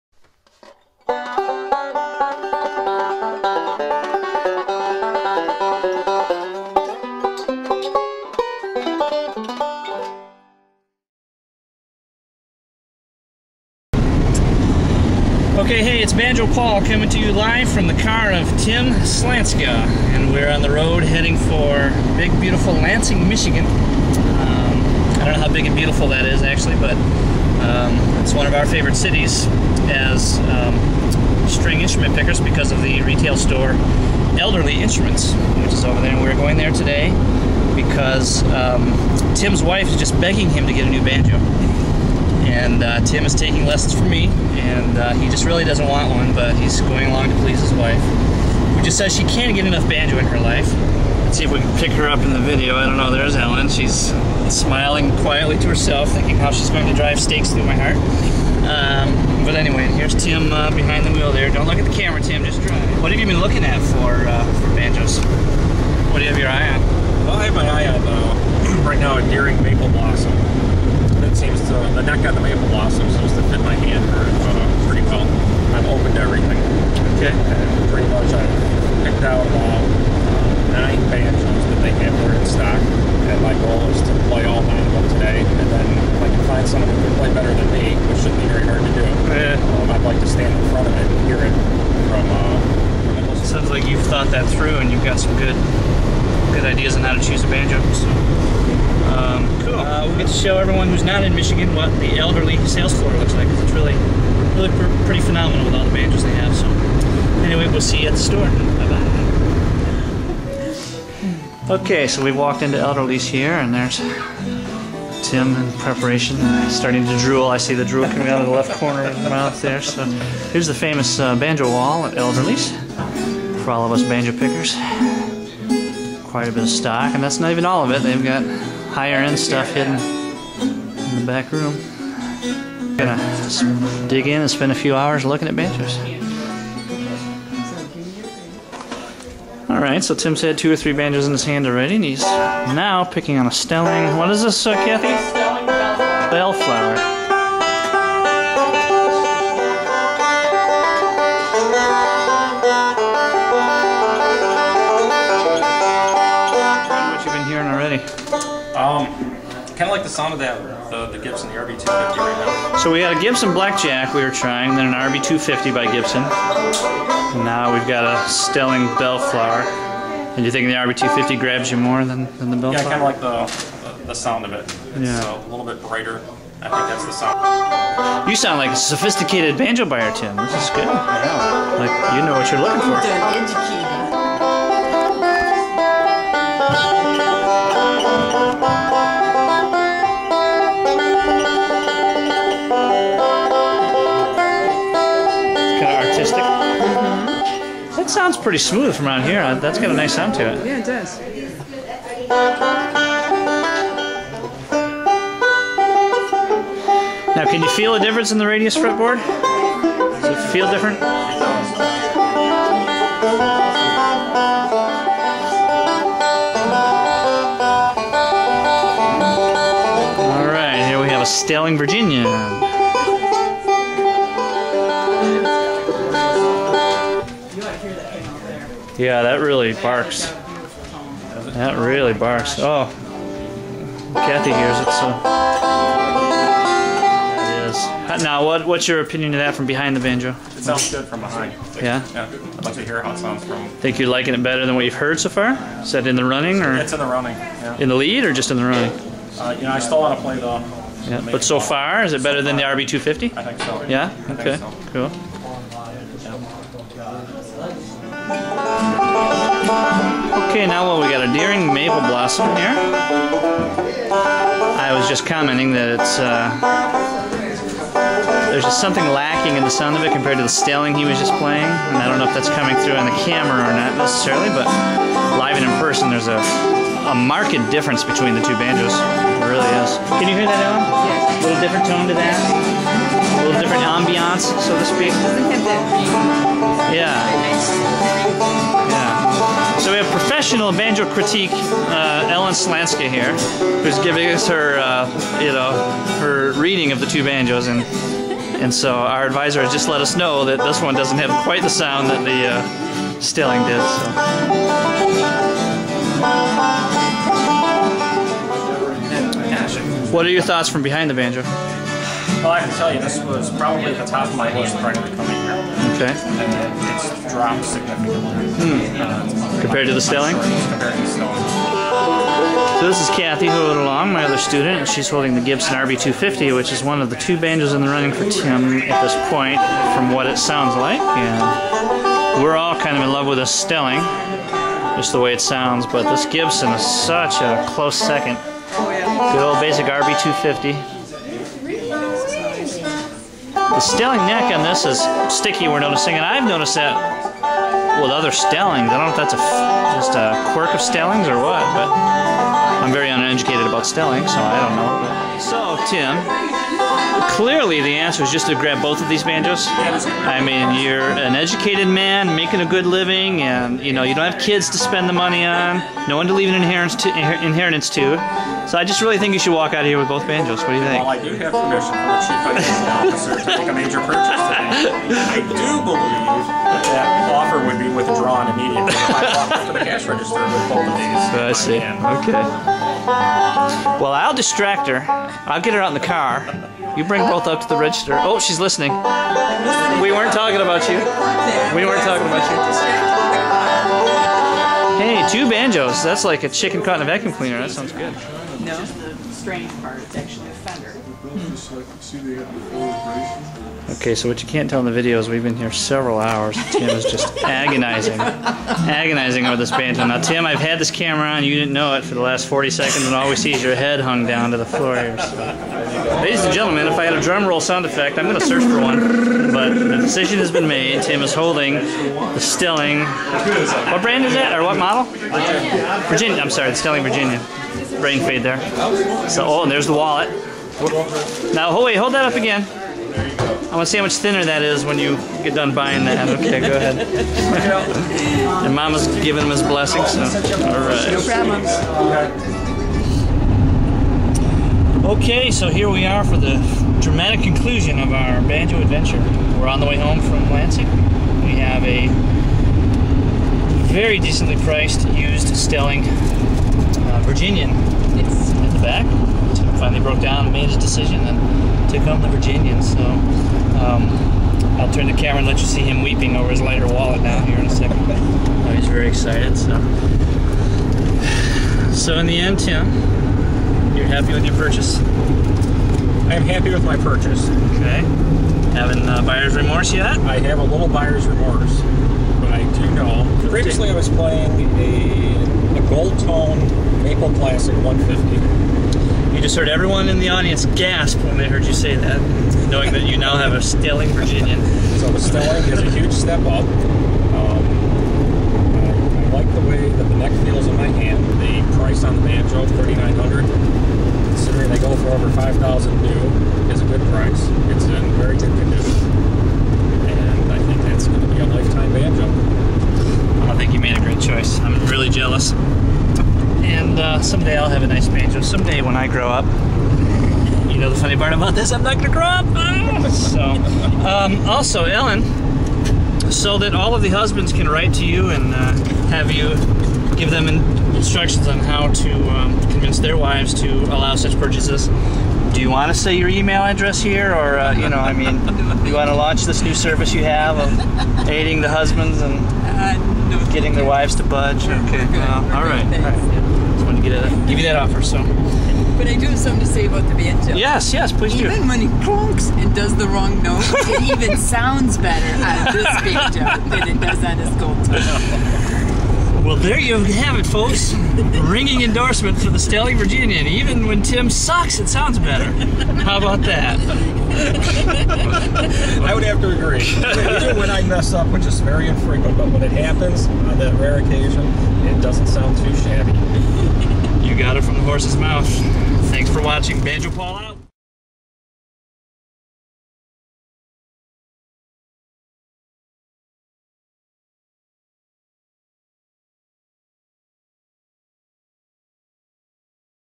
Video blog: Banjo Paul & Tim shop for a banjo!
Join Banjo Paul from Banjos Rule as he and his student Tim go shopping for a new high-end banjo at Elerly Insruments Distributed by Tubemogul.